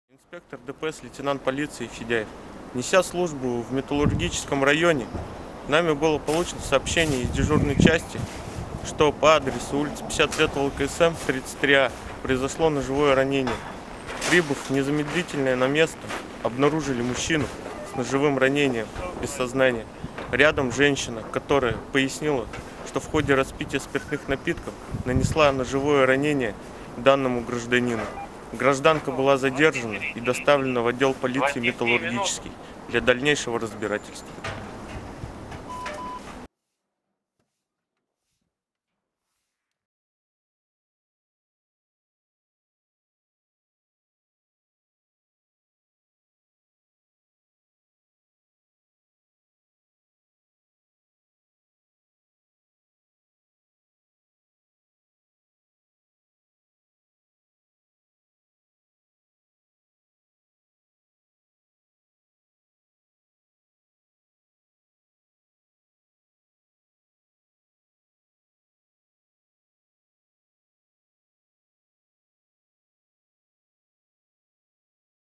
Сотрудники ГИБДД задержали подозреваемую в убийстве.
Сотрудники ГИБДД Челябинска задержали женщину по подозрению в убийстве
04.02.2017 года 17 часов 20 минут от дежурной части отдела полиции «Металлургический» поступила сообщение о том, что  на улице 50 лет ВЛКСМ мужчина получил ножевое ранение в грудь. Ближайшим нарядом полиции, находившимся в районе происшествия, был наряд дорожно-патрульной службы полка ДПС ГИБДД УМВД России по городу Челябинску. Инспекторы незамедлительно прибыли по указанному адресу. В квартире был обнаружен труп мужчины 1962 года рождения с колото-резаной раной, в этой же квартире полицейские задержали женщину 1967 года рождения (супругу погибшего). При выяснении обстоятельств происшествия было установлено, что в ходе распития спиртных напитков между мужем и женой произошел конфликт, в результате которого женщина схватила нож и причинила мужчине ножевое ранение.
На место  была вызвана следственно-оперативная группа, женщина была задержана и доставлена сотрудниками ГИБДД в отдел полиции Металлургического района города Челябинска для дальнейшего разбирательства. По данному факту следственными органами возбуждено уголовное дело по признакам состава преступления, предусмотренного ч. 1 ст. 105 Уголовного кодекса Российской Федерации «Убийство». Санкция данной части статьи предусматривает максимальное наказание в виде лишения свободы на срок до 15 лет.
Подозреваемая в порядке ст. 91 Уголовно-процессуального кодекса Российской Федерации водворена в изолятор временного содержания.